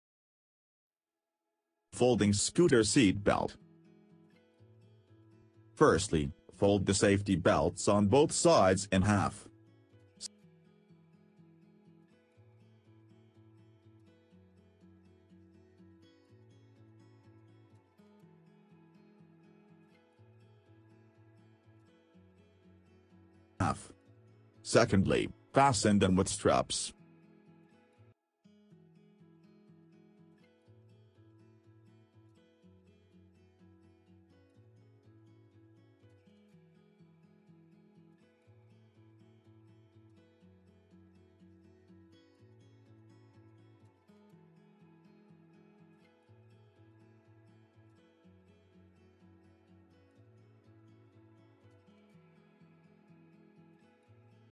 Easy to fold seat belt of electric powered wheelchair ROBOOTER X
Folding Scooter seat belt
1.Fold the safety belts on both sides in half
2.Fasten them with straps

If you are interested, please contact the BBR.